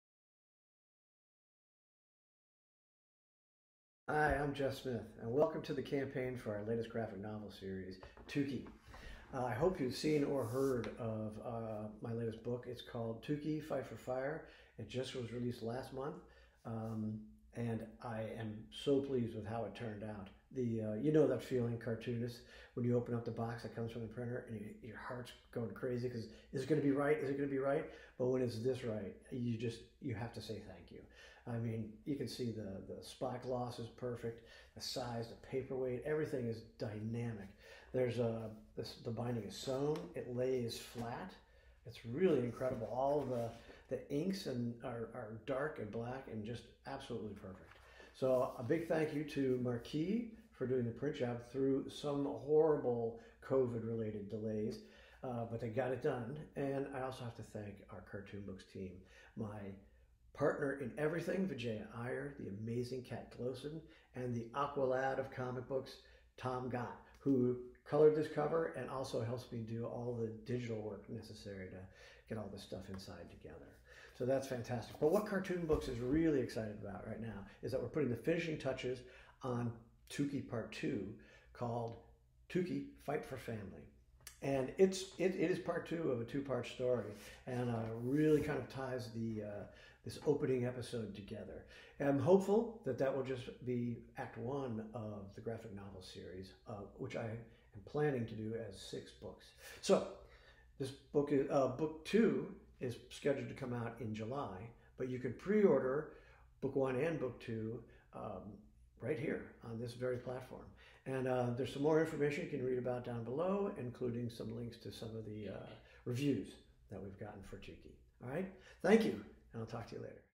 Jeff Smith's TUKI Fight for Family
Welcome! We are launching our crowdfunding campaign for TUKI: Fight for Family - we're glad you're here!

Two million years ago more than one species of human existed at the same time,  but only one could control FIRE.

At the dawn of humanity, when multiple species of humans existed at the same time, three lost children and two missing links fall in with a mysterious traveler named Tuki who carries with him the secret of Fire.  A rival species, who hunt and kill anyone found using fire, gets word of the small band of misfits, and the new companions find themselves under attack!